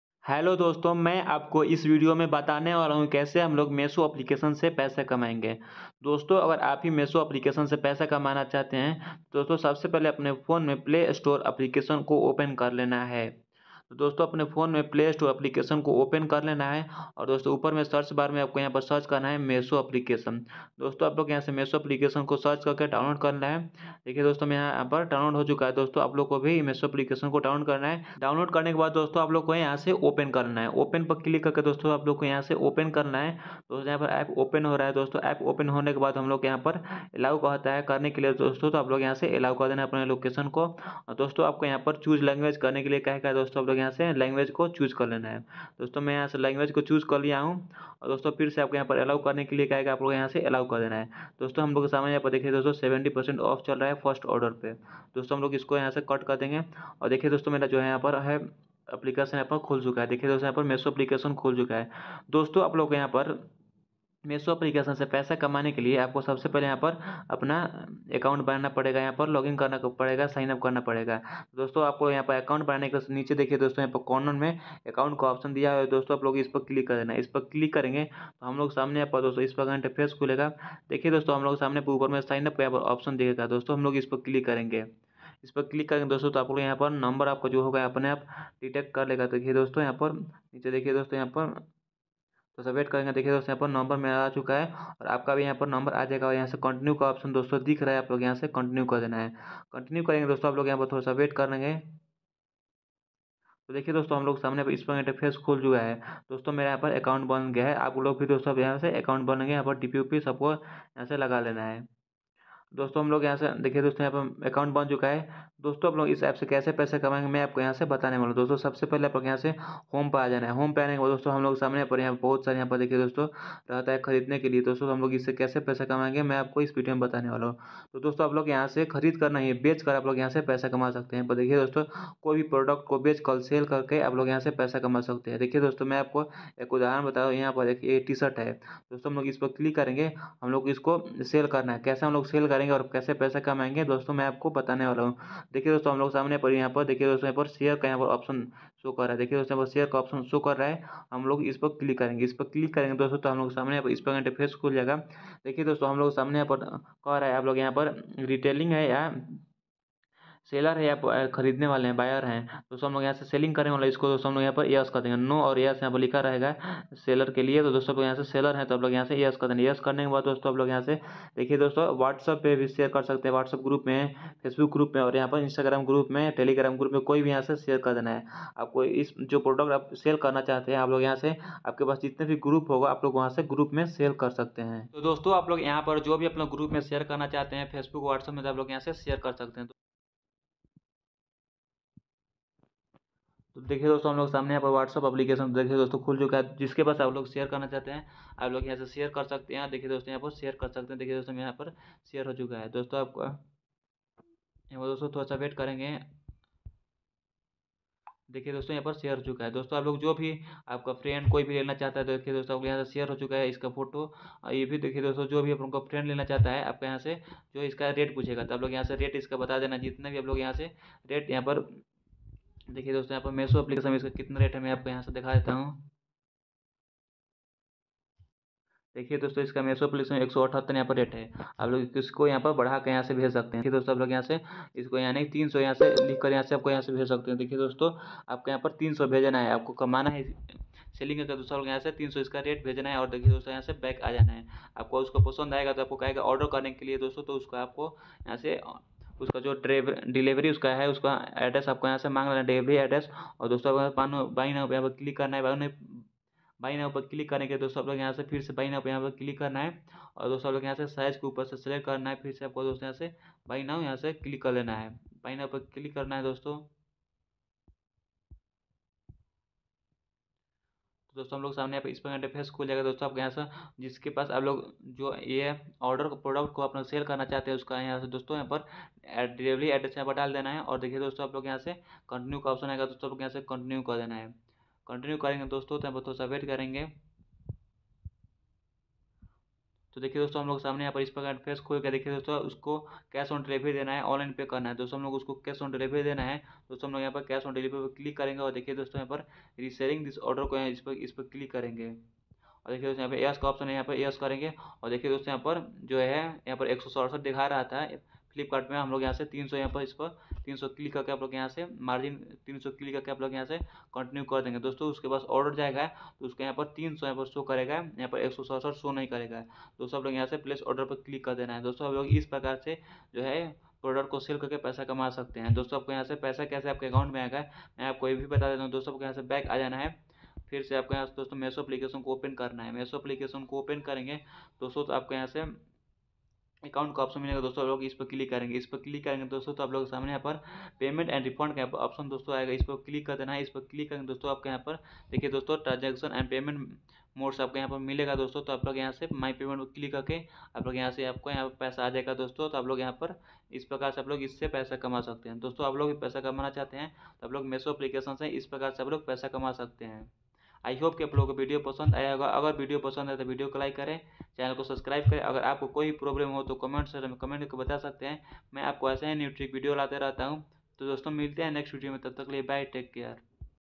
Meesho se paisa kaise kamaye 2024 !! meesho app se paisa kaise kamaye !!
Hello guys welcome to channel , Dosto mai aapko is video me bataya hu ki meesho app se paisa kaise kamaye koi bhi Product ko sell kar ke .